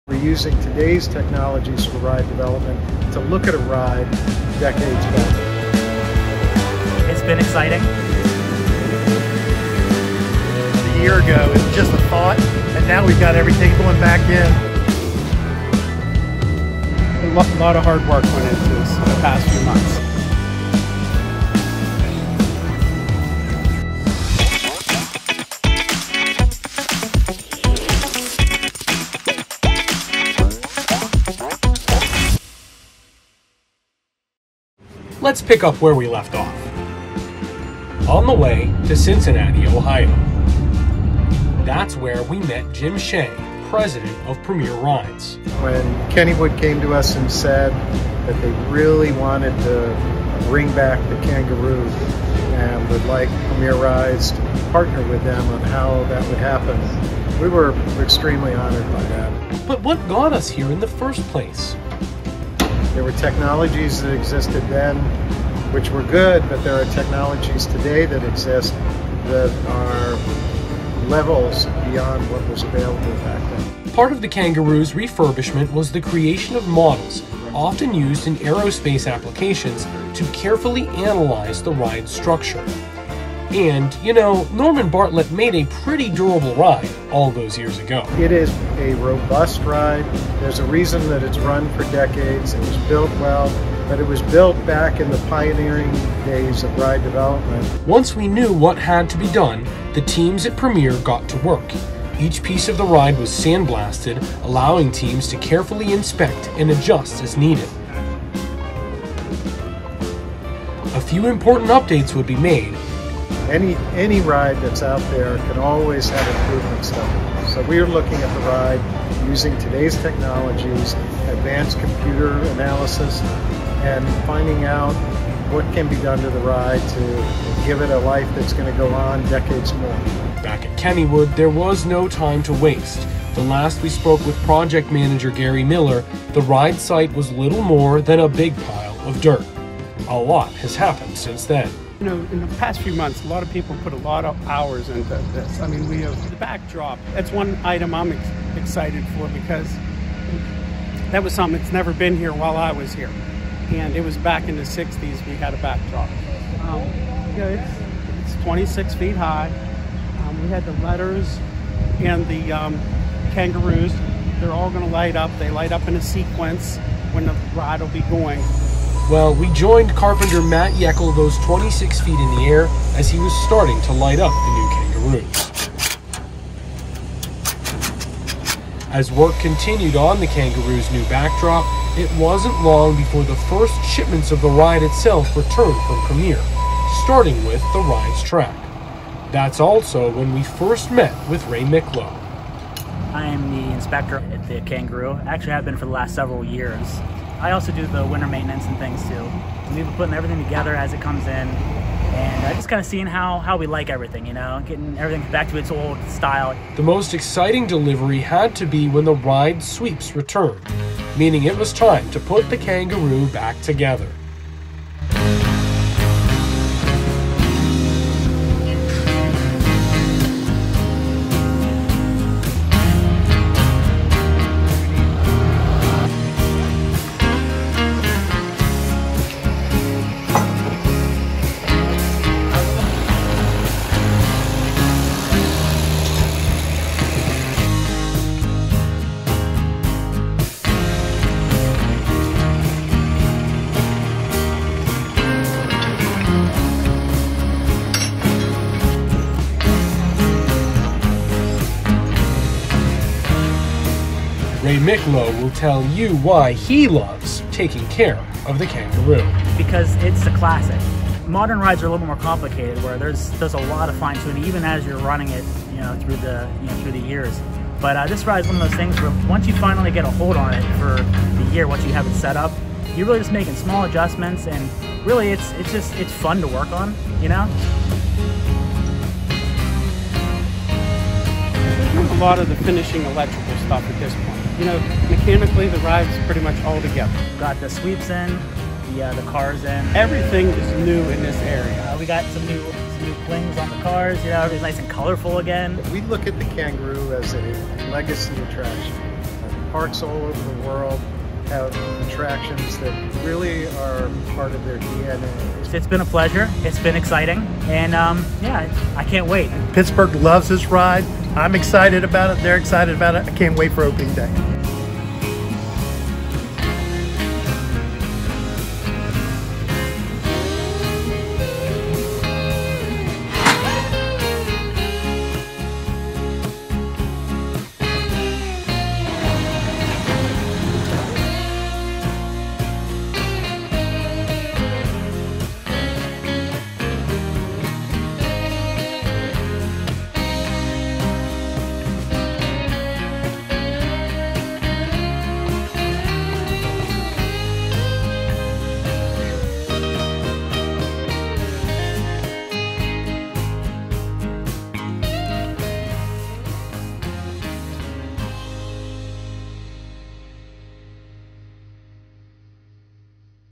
Can't Stop the Hop, Part 2 | The Refurb Phase | Kangaroo Returns at Kennywood
Part 2 of Can't Stop the Hop is here! Go behind the scenes of the Kangaroo's refurbishment, from engineering at Premier Rides to welcoming the attraction back to Kennywood. We can't wait for the first rides very, very soon!